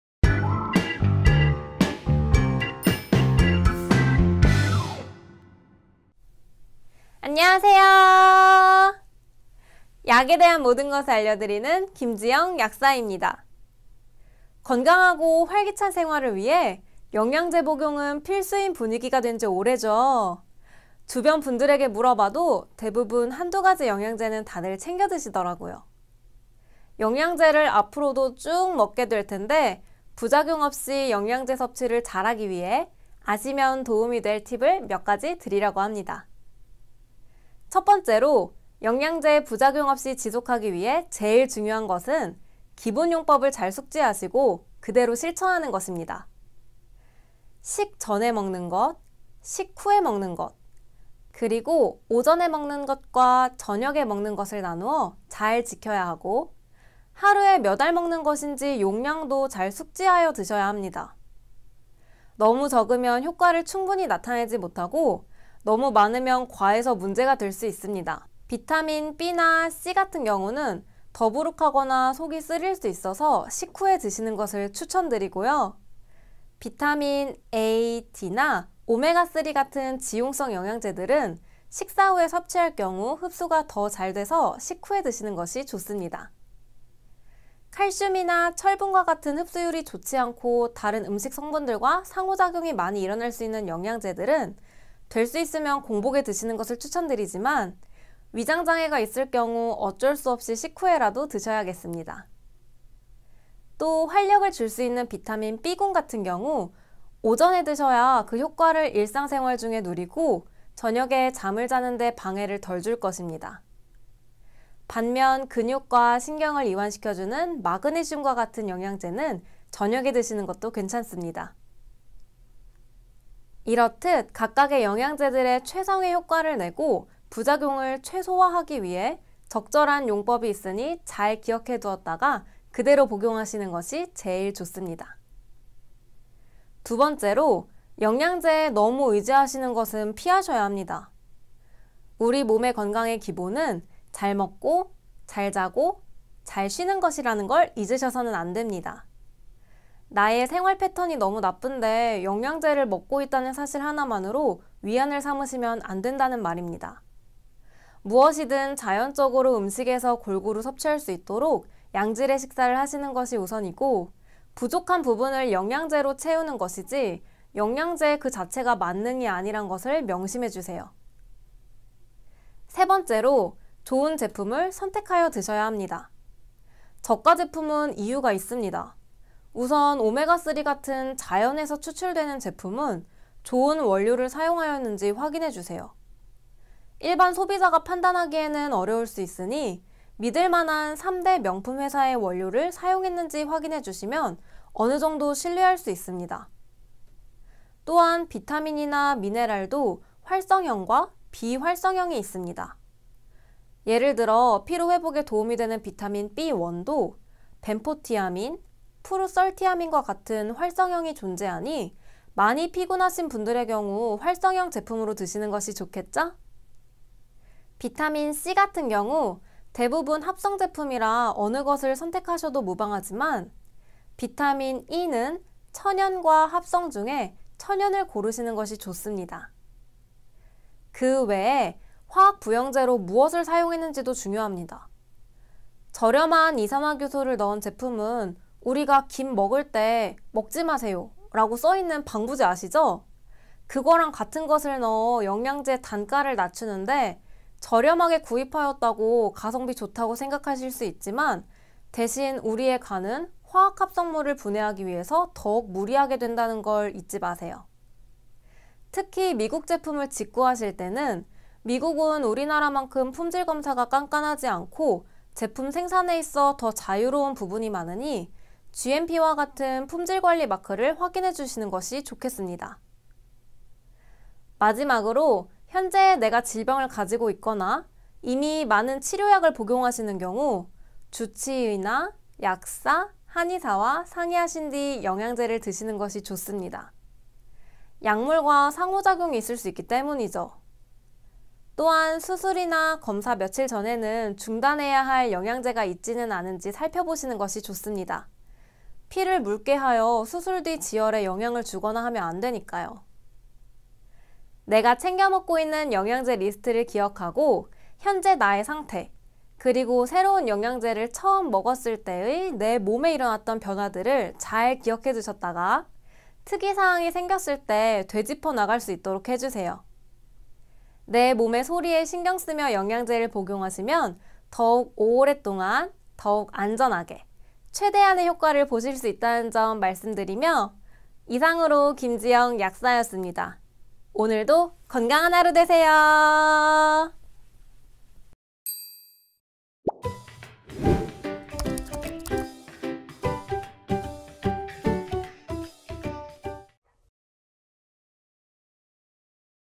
부작용 없이 영영제 섭취를 하려면 ?
오늘은 김지영 약사님과 함께 부작용 없이 영영제를 섭취하는 방법에 대해서 알아보는 시간을 갖도록 하겠습니다 ! 꼭 확인하셔서 올바르게 선택하세요

출연: 김지영 약사님

00:00 인사
00:26 부작용 없이 영양제 섭취하는법

관련 기사: 오메가3 고를 때 ‘반드시’ 확인해야 할 점 3가지